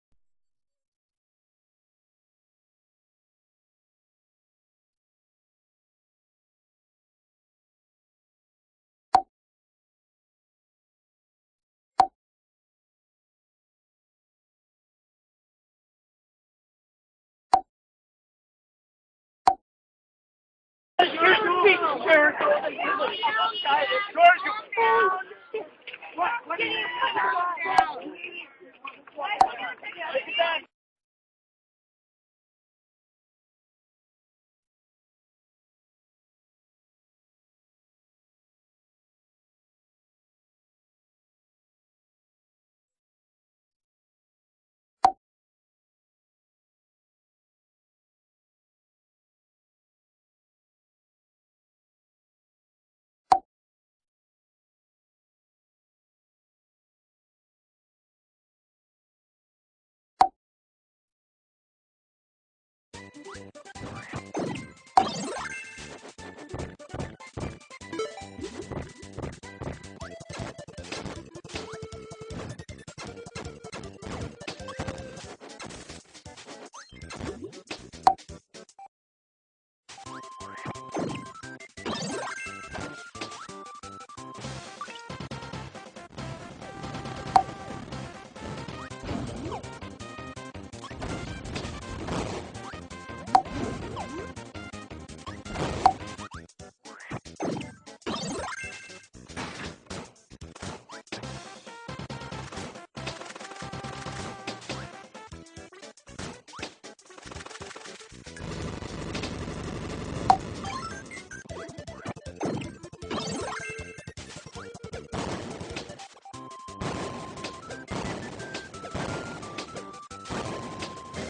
Youtube for TV!
This is a Youtube made for TV. Specificly made for Wii and PS3. Sorry if you hear some popping sounds, I was in a chat room.